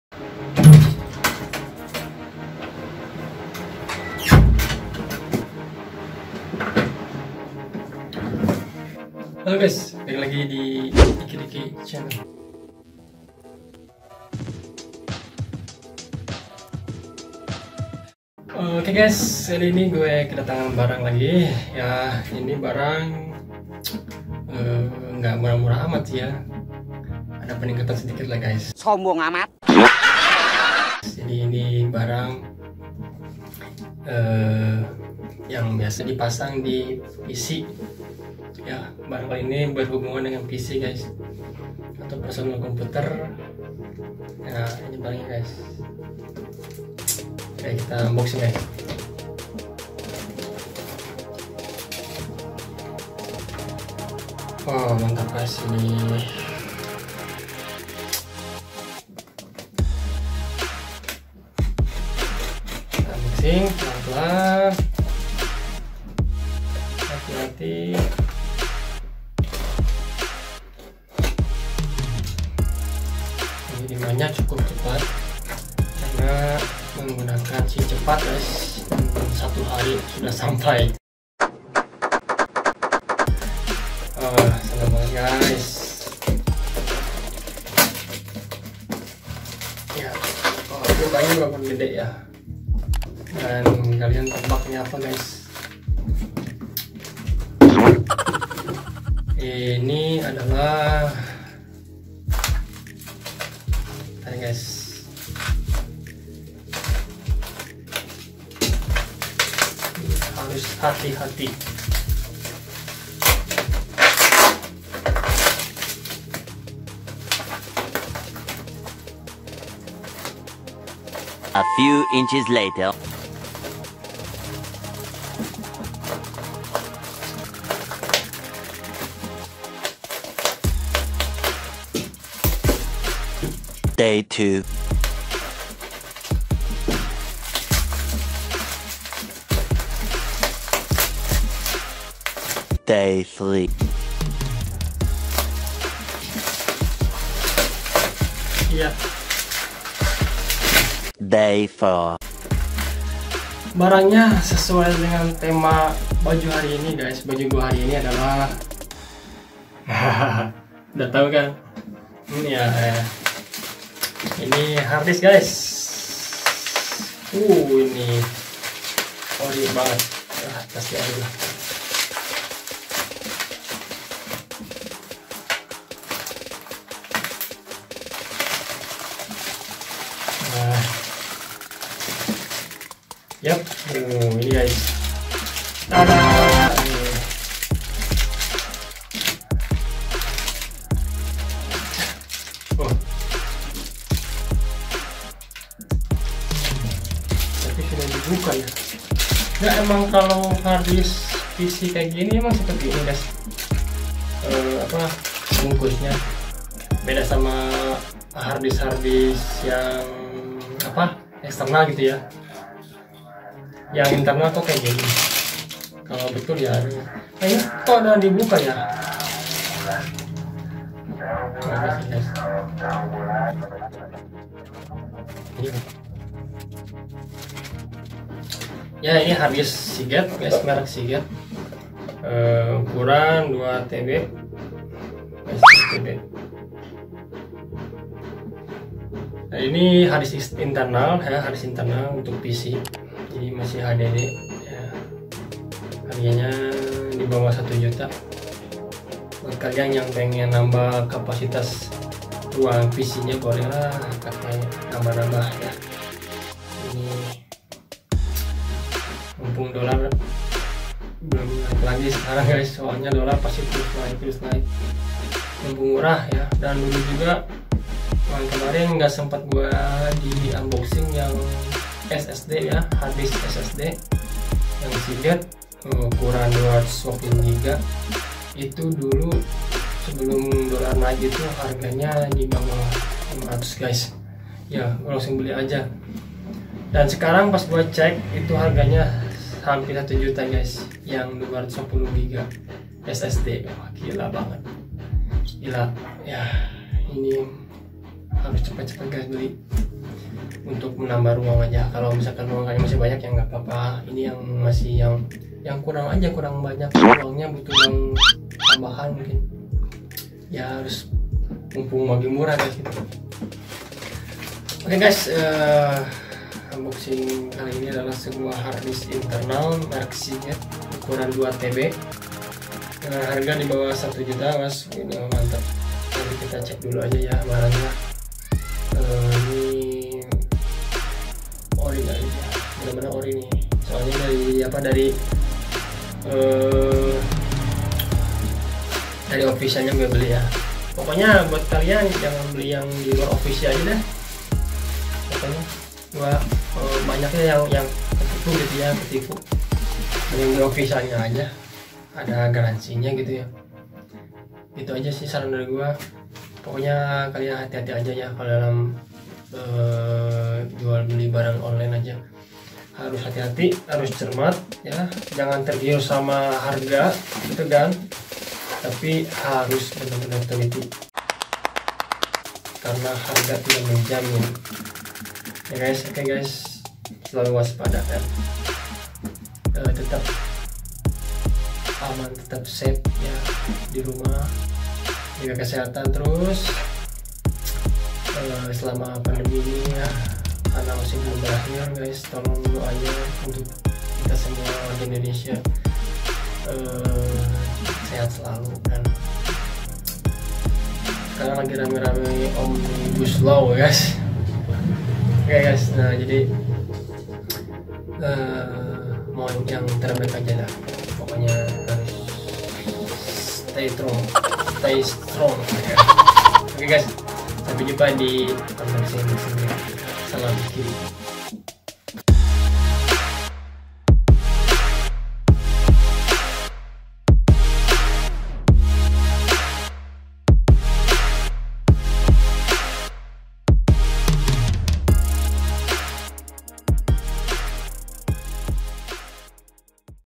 #47 UNBOXING HARDISK INTERNAL 2TB HDD MERK SEAGATE
Terima kasih sebelumnya & untuk request/mendukung channel ini silahkan klik link dibawah ini guys. :-)

Oke guys balik lagi di ykyriky channel, kali ini gua akan unboxing sebuah Hardisk Internal HDD buat PC merk Seagate dengan kapasitas 2TB atau sekitar 2000 GB. silahkan cek aja videonya brosis.
Semoga Bermanfaat.

Sampai jumpa di video unboxing-unboxing selanjutnya.
Salam ykyriky